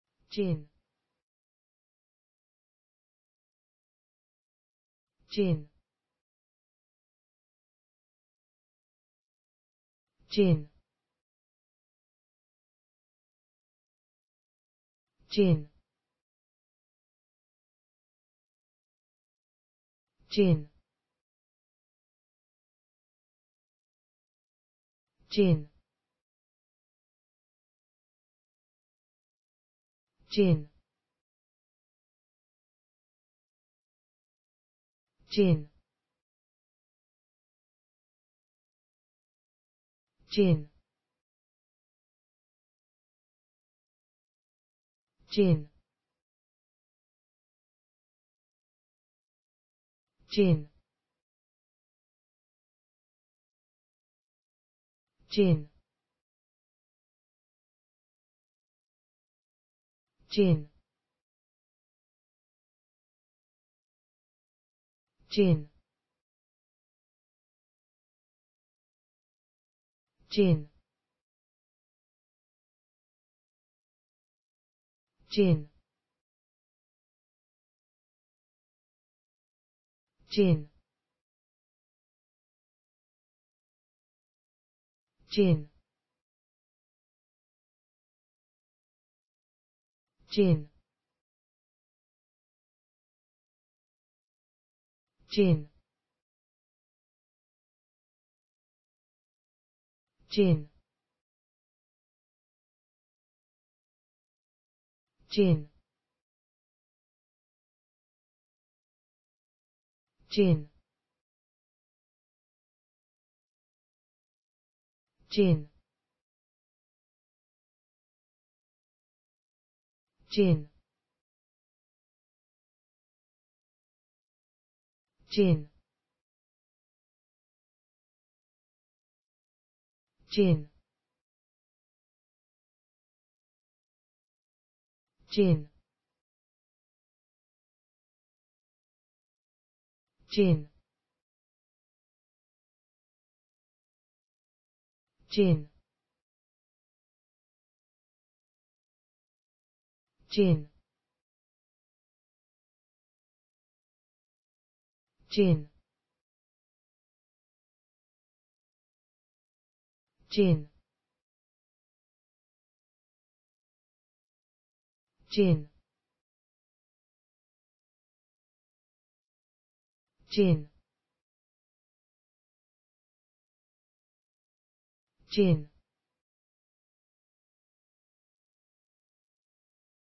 djin. How to pronounce the english word djin .Start with D. Learn from me.
Follow me!Learn from me how to pronounce the english word djin.This video shows you how to pronounce the English word djin .